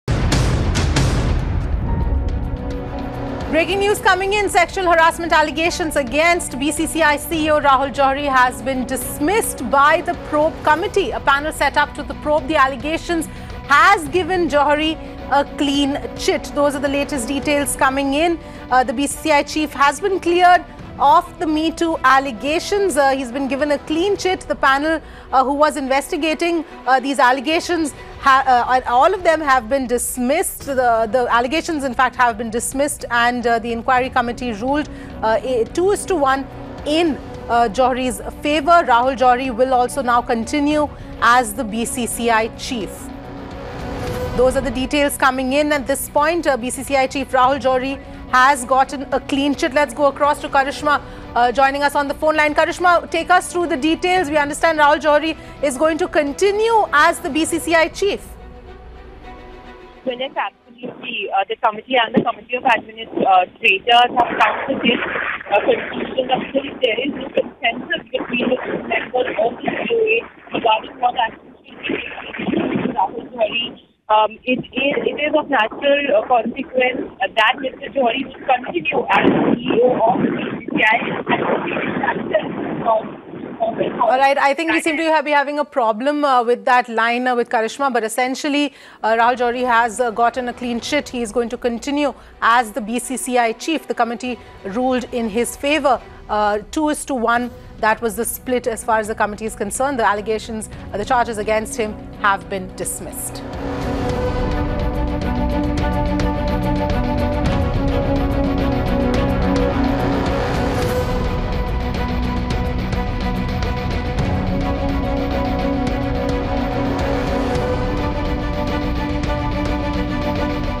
BCCI CEO Rahul Johri cleared of sexual harassment charges
BCCI CEO Rahul Johri was on Wednesday cleared of sexual harassment allegations which dismissed the charges levelled by at least two women as "mischievous and fabricated". Listen in!